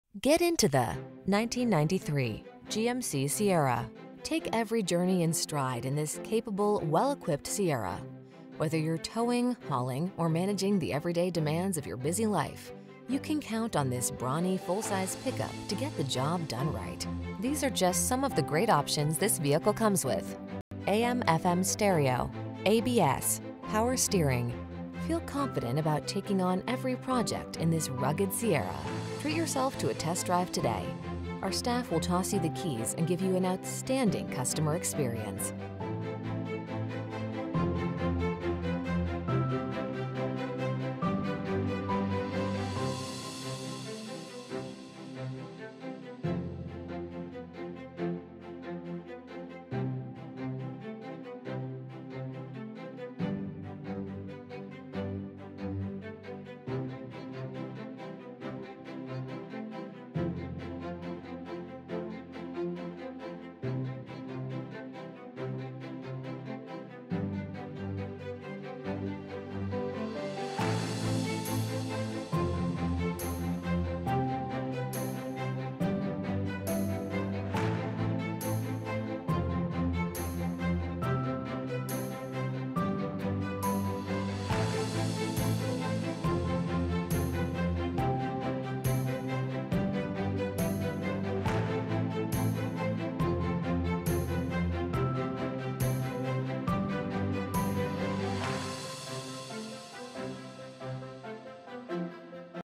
1993 GMC C/K_1500 Davison, Flint Grand Blanc, Lapeer, Burton 6-29253DK
Used 1993 GMC C/K_1500  available in 800 N. State Rd., Davison, Michigan at Hank Graff Chevrolet. Servicing the Davison, Flint, Grand Blanc, Lapeer, Burton, MI area.

C/K_1500
1993 GMC C/K_1500  - Stock#: 6-29253DK

800 N. State Rd

Recent Arrival! 4.3L V6 12V RWD.brbrbrRWD 4.3L V6 12V
4 Speakers|AM/FM radio|Power steering|ABS brakes|Front anti-roll bar|Bumpers: chrome|Voltmeter